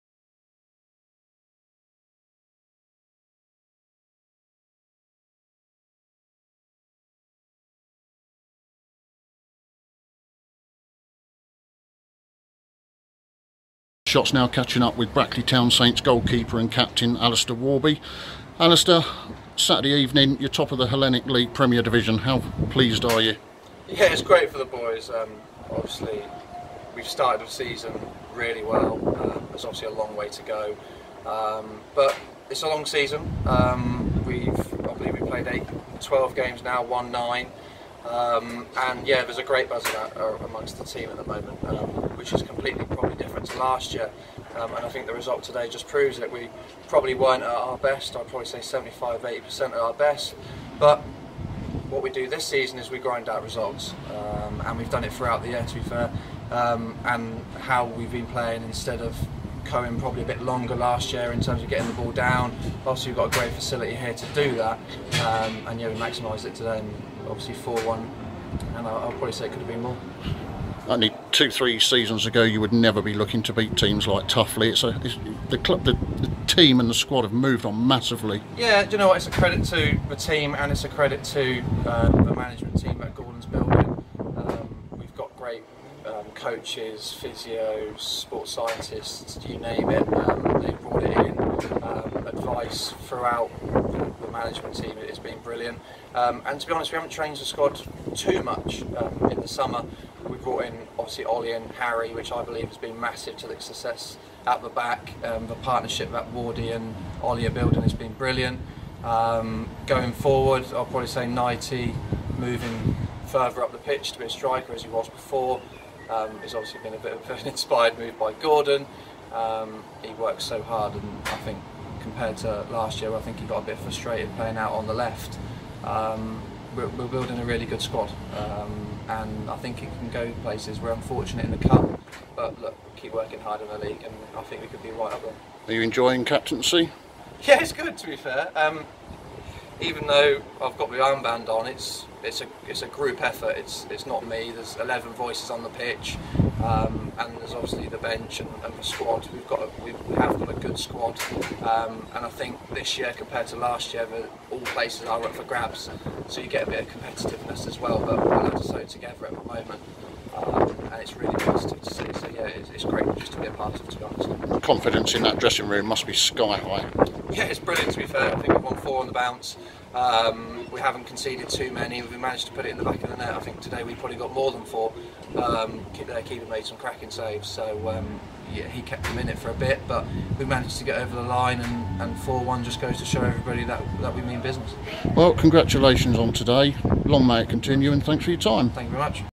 Alastair Worby 13 10 2018
Brackley Town Saints keeper & skipper Alastair Worby after 4-1 win over Tuffley Rovers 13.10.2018